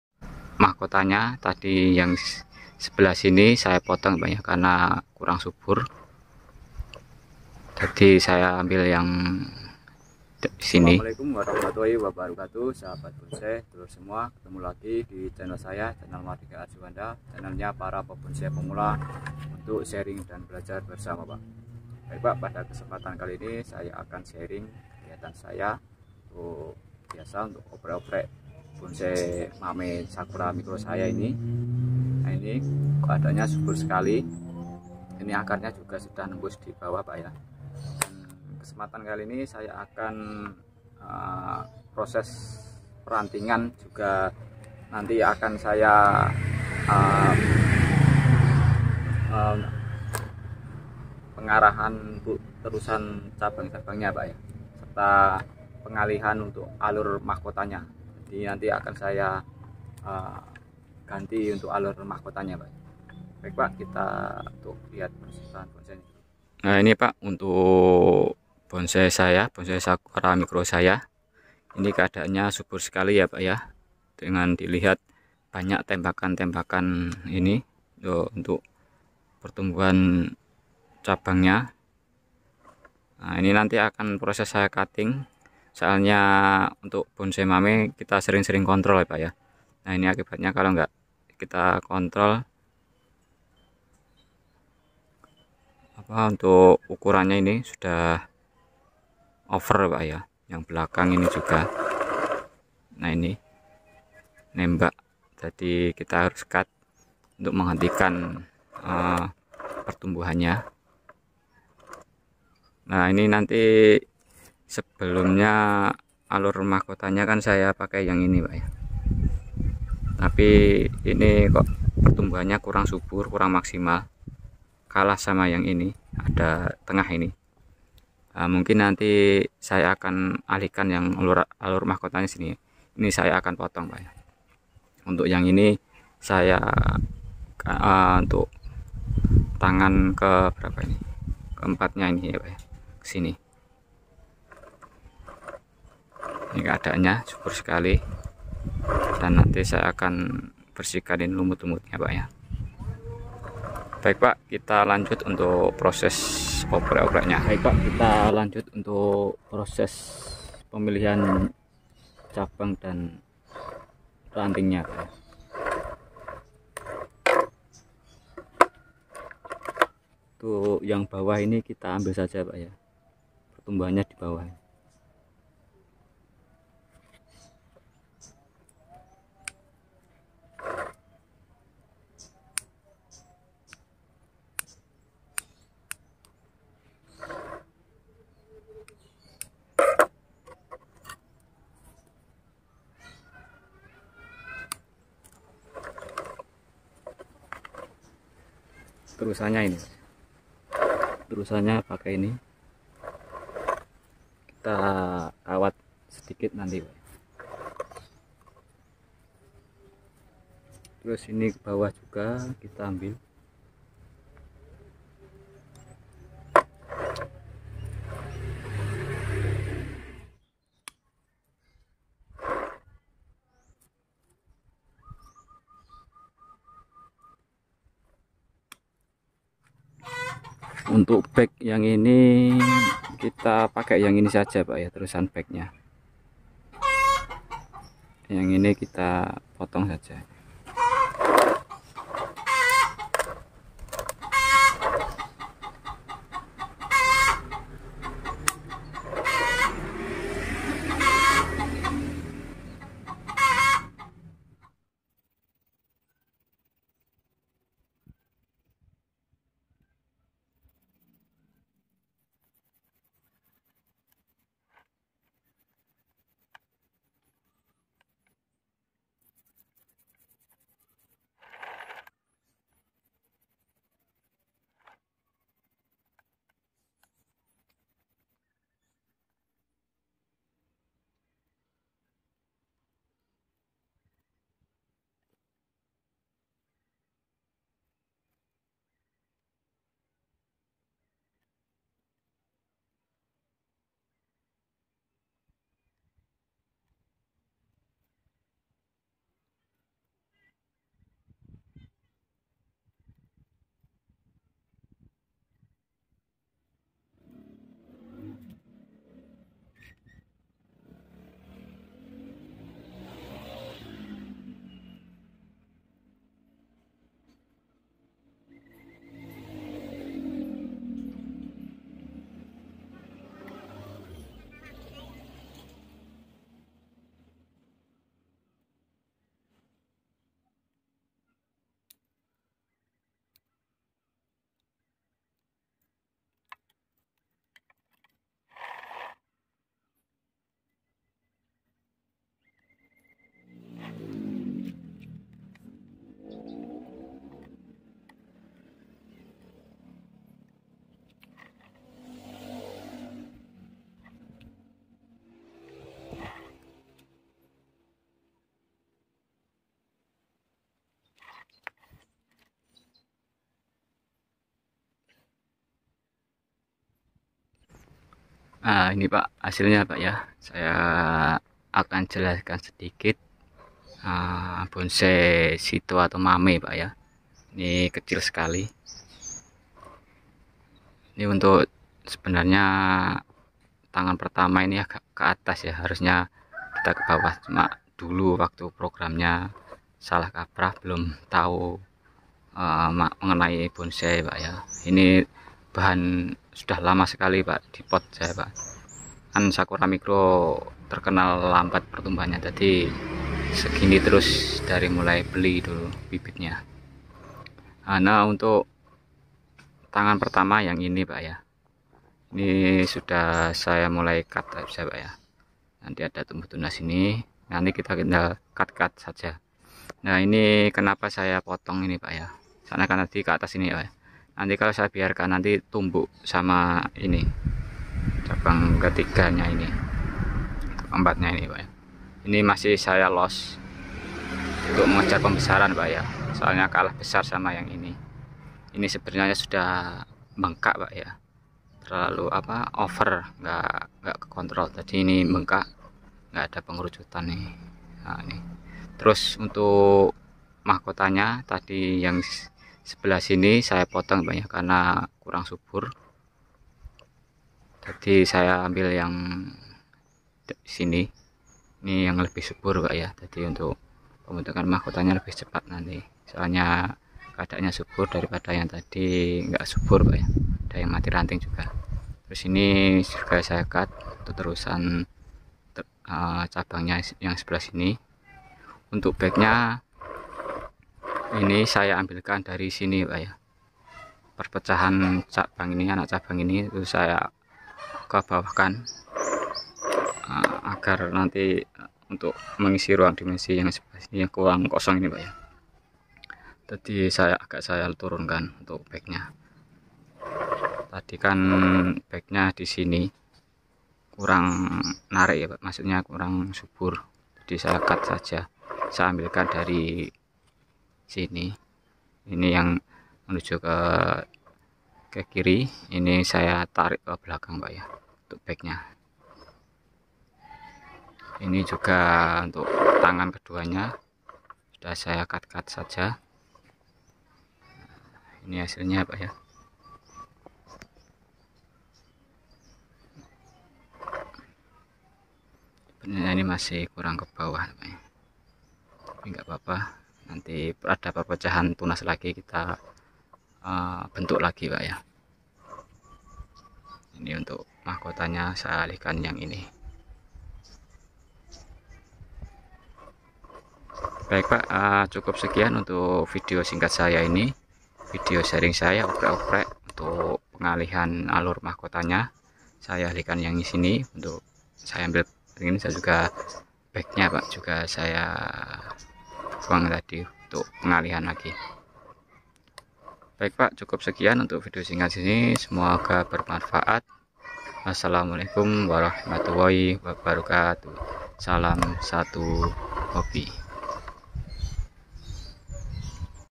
MERUBAH TERUSAN ALUR MAHKOTA PADA BONSAI "SAKURA MIKRO"|| MEMBUAT BONSAI SAKURA MIKRO
Hallo Sahabat Bonsai...
Seni Bonsai adalah merupakan seni hidup maksutnya dalam berseni bonsai atau membuat bonsai benda yang kita buat adalah hidup yakni dimaksut adalah pohon, dimana setiap waktu pasti ada perubahan yakni bentuk dari pohon yang dibuat bonsai tersebut karena dengan seiringnya waktu.
Contohnya ketika bonsai kita karena tidak terawat ataupun pada saat nutrisi pada media tanam habis, akan mengalami mati cabang ataupun mati ranting, sehingga kita harus melakukan inovasi atau perubahan atau kita harus melakukan reposisi pada bonsai tersebut.
Pada kesempatan kali ini saya akan berbagi kegiatan saya yakni mengenai merubah pada alur terusan mahkota pda bonsai sakura mikro saya, ini saya lakukan karena alur mahkota sebelumnya mengalami perlambatan pertumbuhan, Sehingga saya melakukan perubahan alur mahkota pada bonsai saya tersebut.
Untuk lebih jelasnya bisa kawan-kawan simak videonya, dan semoga bermanfaat.
Terima kasih, Salam Satu Hobi.